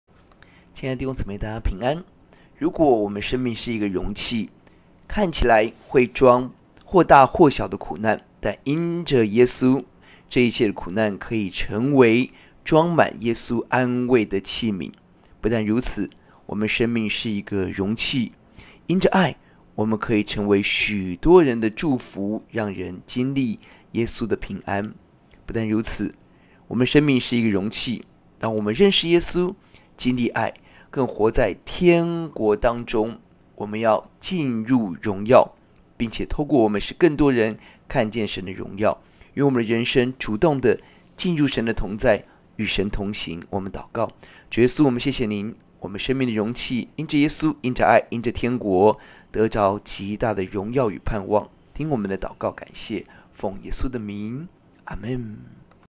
[信息一分鐘] 生命的容器
講員：徐坤靖牧師 貴格會合一堂

哈2:14 認識耶和華榮耀的知識要充滿遍地，好像水充滿洋海一般。

该视频由BUMIPUTRA在Pixabay上发布

该视频由anncapictures在Pixabay上发布

该视频由Moshe Harosh在Pixabay上发布